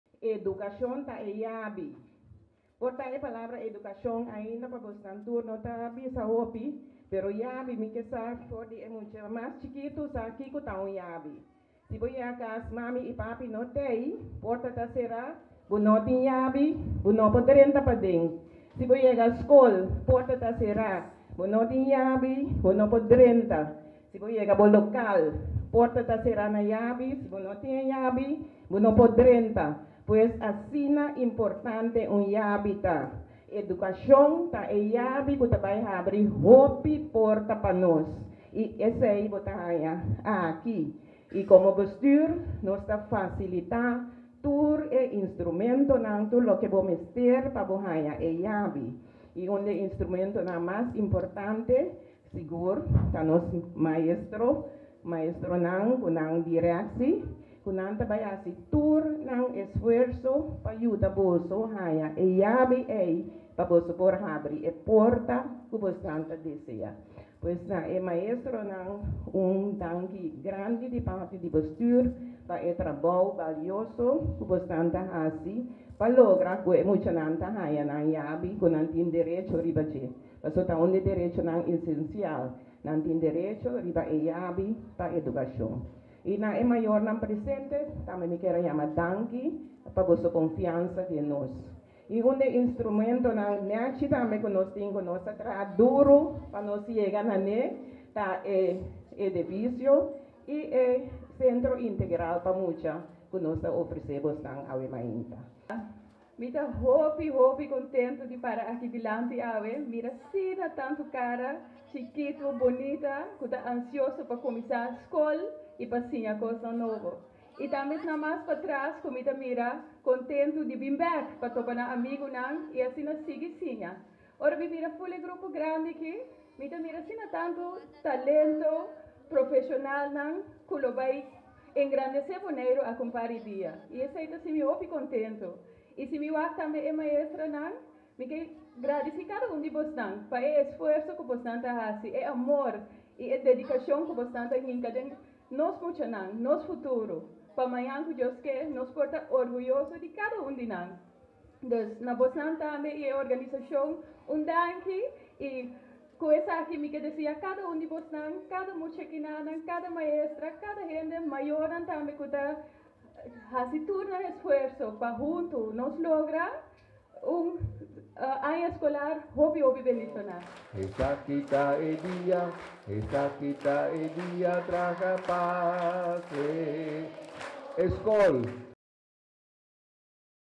2| Kolegio Kristu Bon Wardador a desvela logo nobo.
Diputado Craane i direktor di skol sra. Nilva Wout tambe tabata presente i a enkurasha e muchanan pa e aña eskolar nobo 2023.